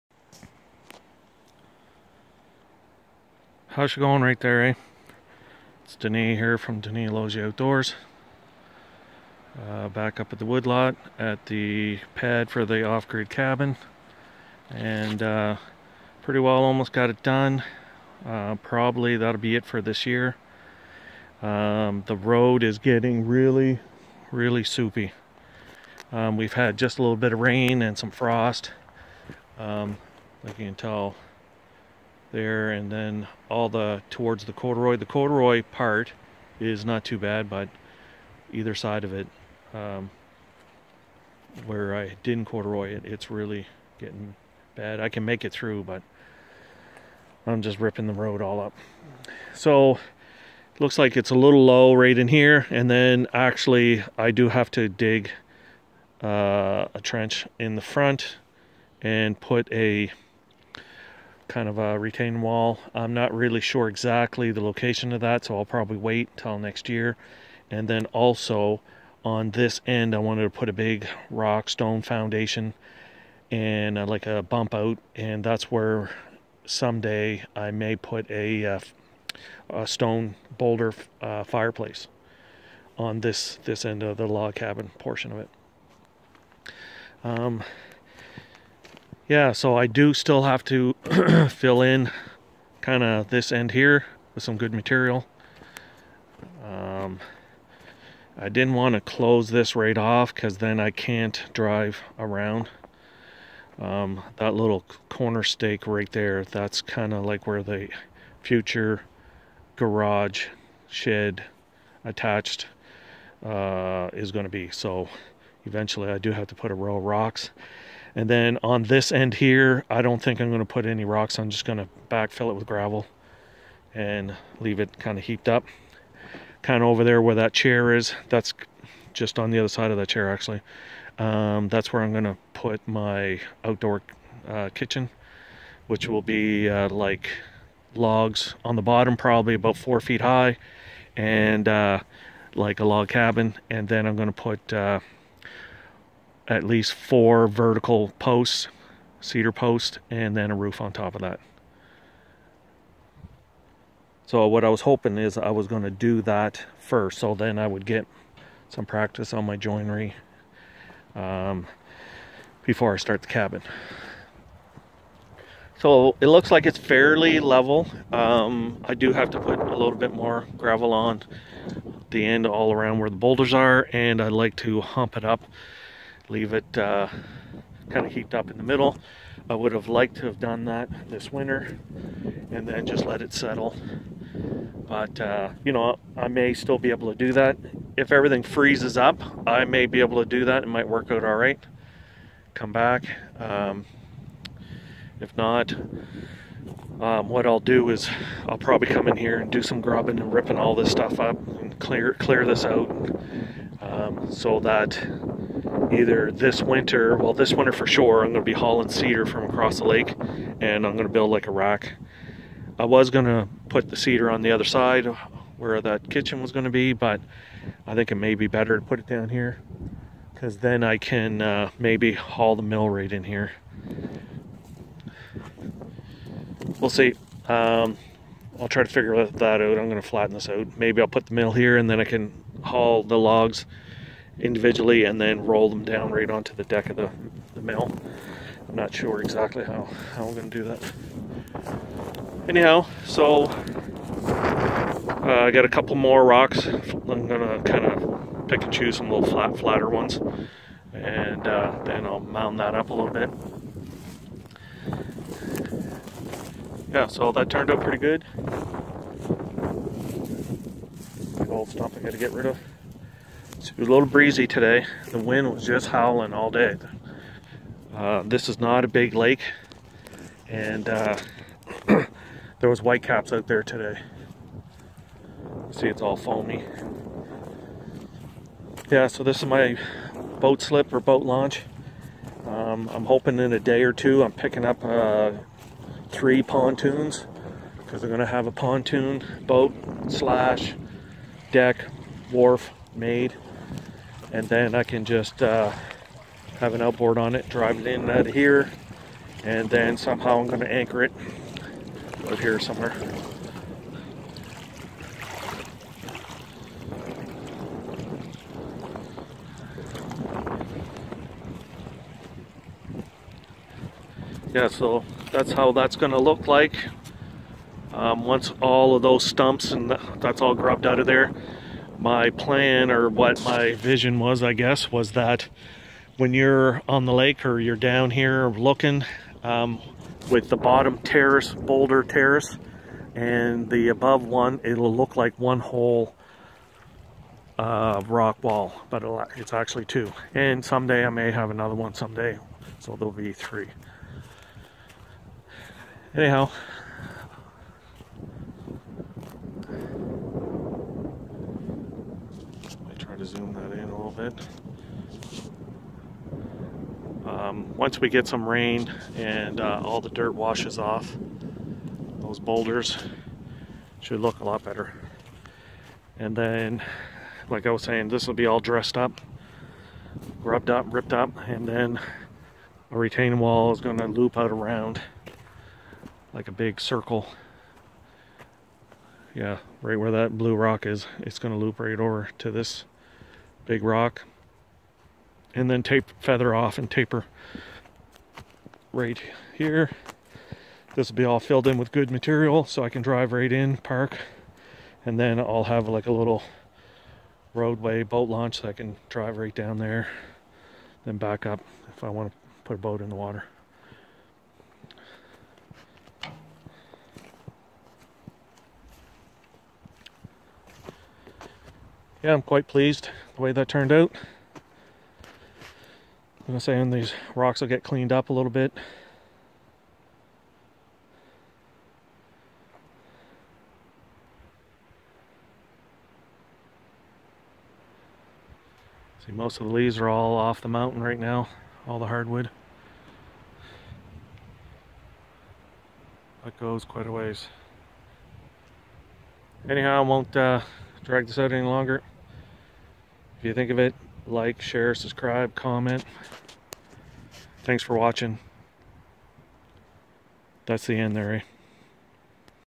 Off grid cabin base is done for the year
Completed backfilling cabin base for
 the year